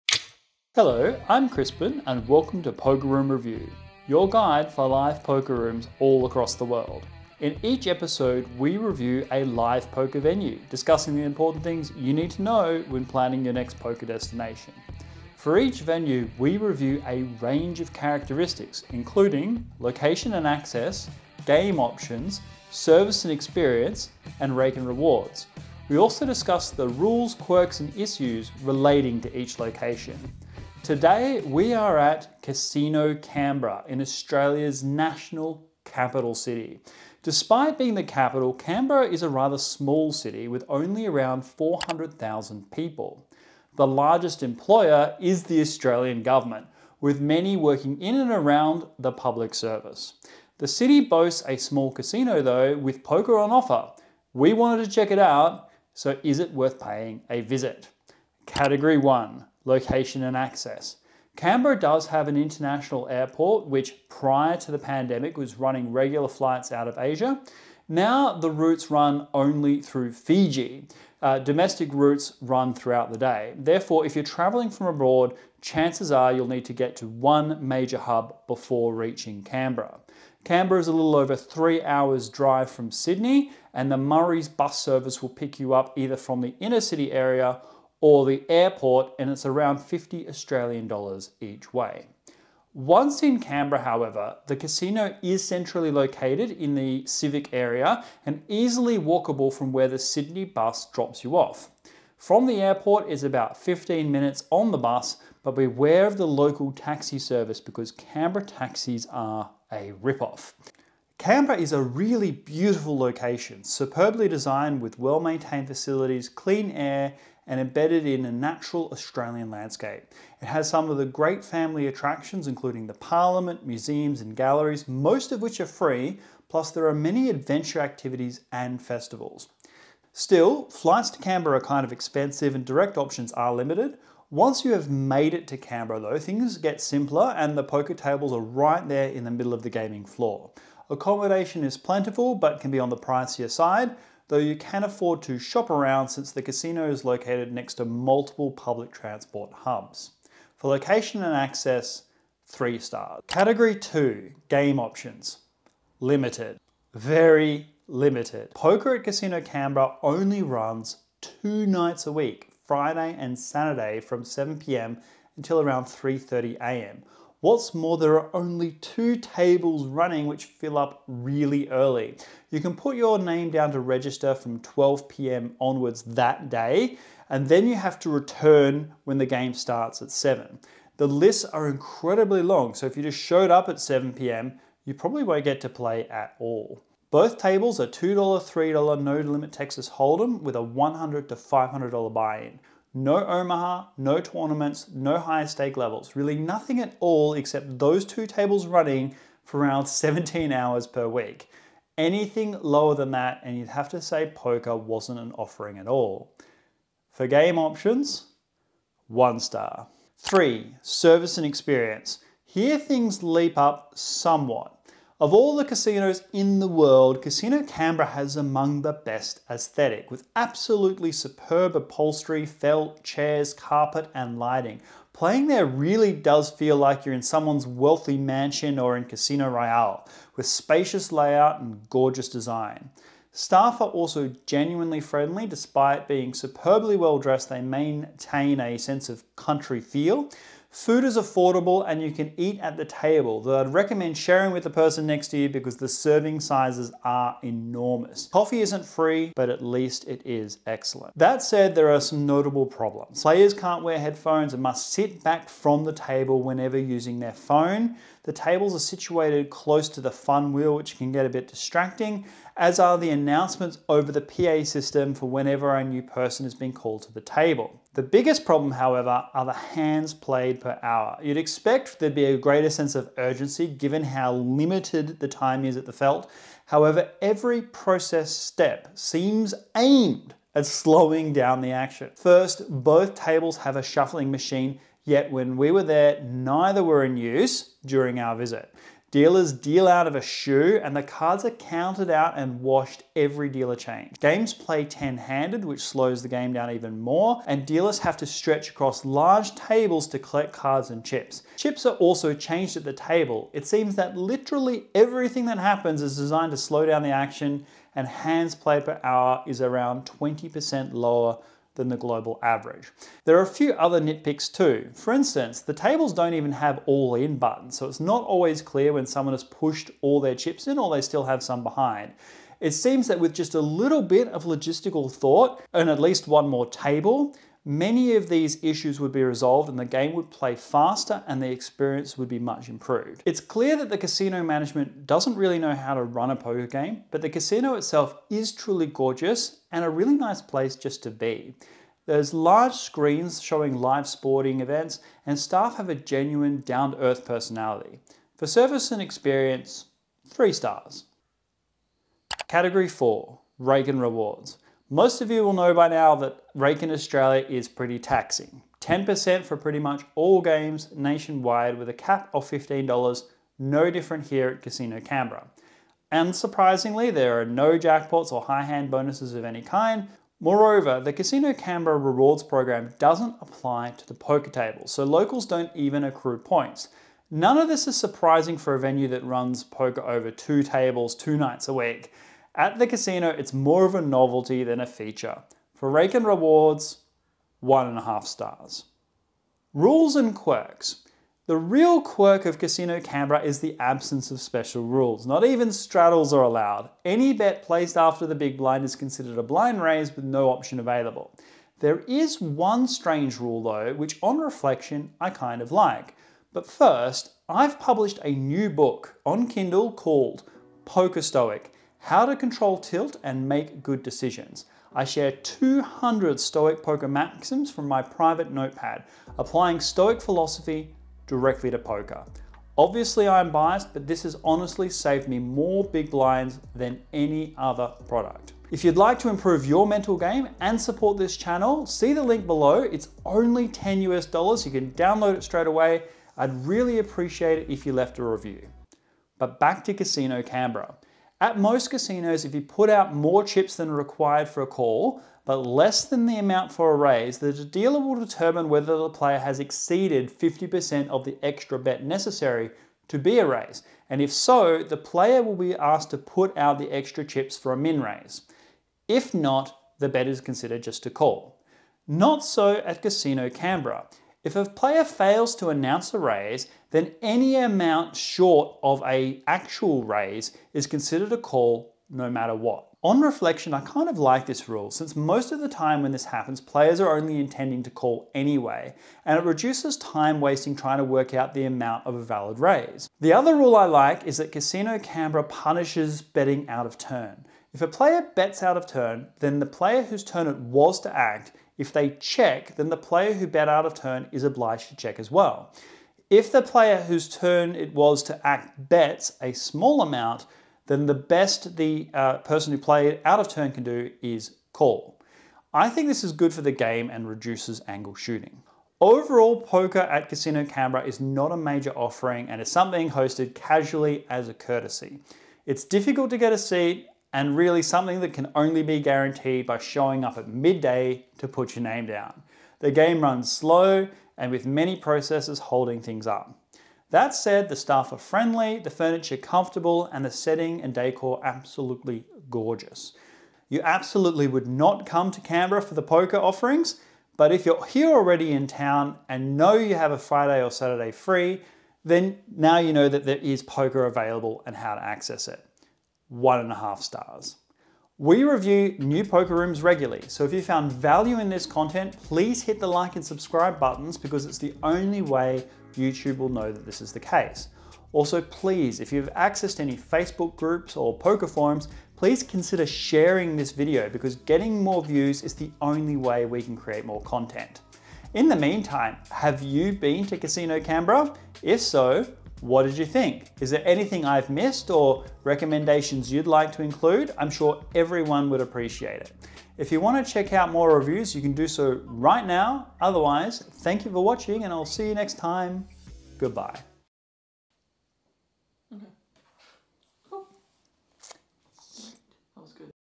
Casino Canberra, Australia - Poker Room Review
Today we review Casino Canberra in Australia.

Canberra is Australia's capital city with national galleries and museums. Normally a quiet place known for its festivals and government buildings, Canberra also has a casino that offers poker (sort of). We go there to play, so how does poker measure up?

New Book!
Poker Stoic: How to Control Tilt & Make Good Decisions
Only $9.99 USD $5 CASHBACK FOR VIEWERS WHO REVIEW
0:00 Introduction
0:58 Location and Access
2:33 Game Options
3:32 Service and Experience
5:51 Rake and Rewards
6:30 Rules and Quirks